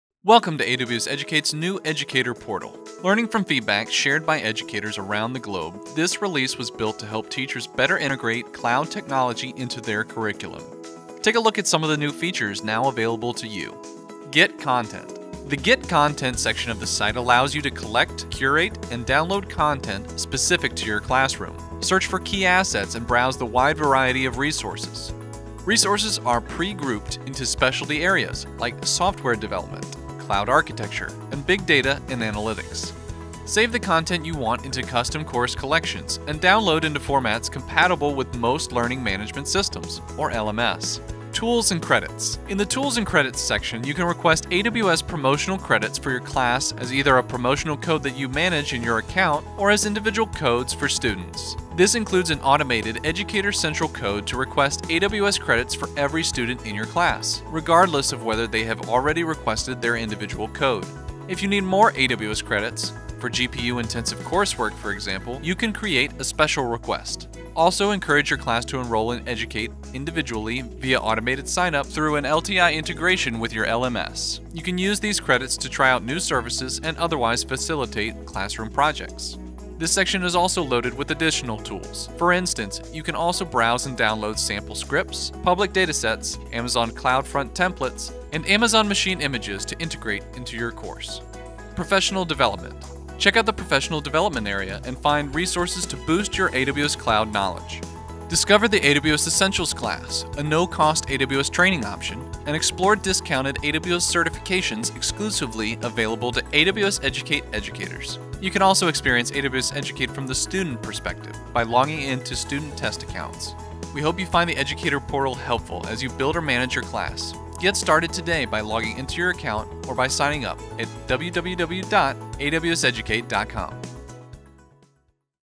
The AWS Educate Educator Portal
The AWS Educate Educator Portal is designed exclusively for educators to get the most out of the AWS Educate experience. Learning from feedback shared by educators around the globe, we’ve built this release to help teachers better integrate cloud technology into their curriculum.

New features include:

- Enhanced ability to collect, curate, and download content specific to your classroom
- Learning Tools Interoperability (LTI) for integration with your LMS
- Easily request AWS Promotional Credits for your class with teacher central codes
- New Professional Development resources to boost your AWS Cloud knowledge

We hope you enjoy this release and find it useful as you build and manage a class environment.